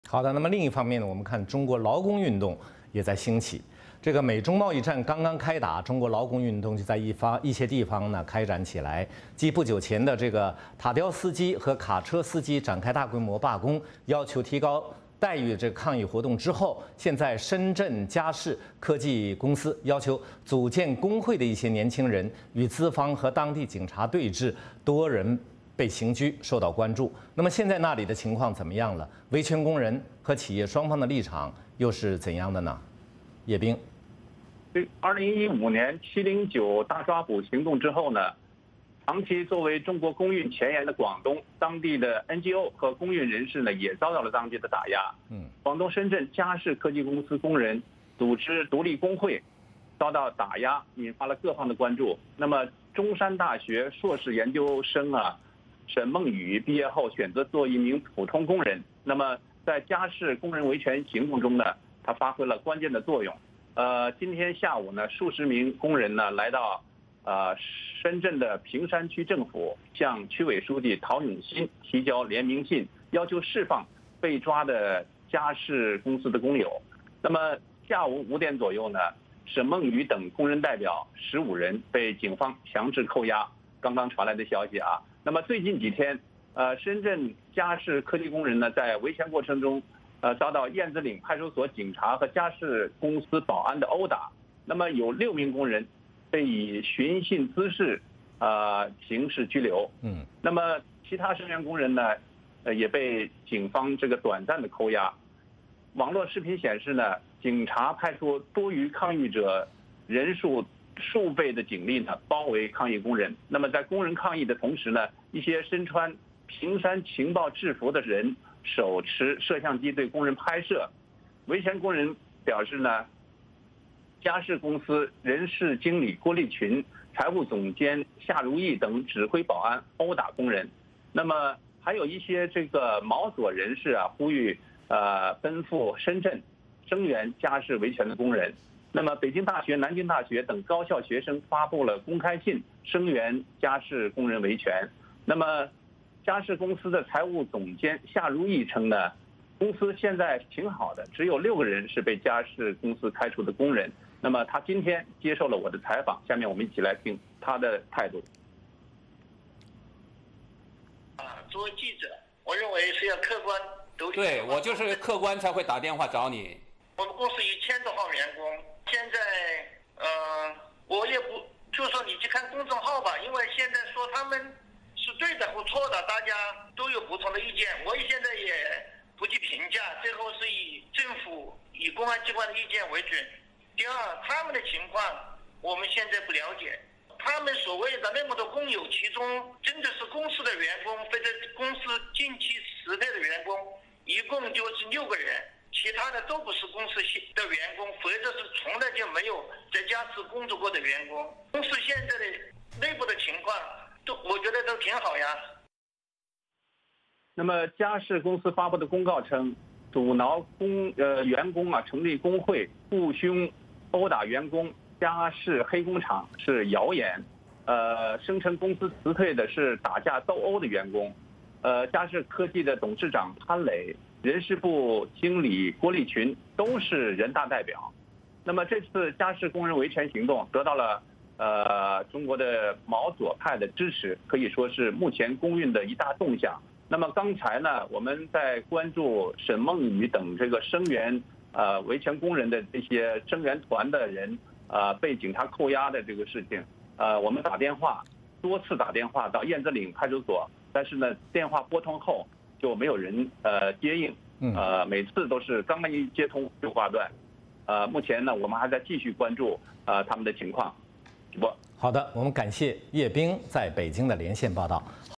VOA连线(叶兵)：深圳佳士劳工维权运动引发多方关注
深圳佳士科技公司工人争取劳工权益的维权抗争持续进行，目前已经获得海内外上千人联名声援。美国之音驻北京记者叶兵带您了解有关情况。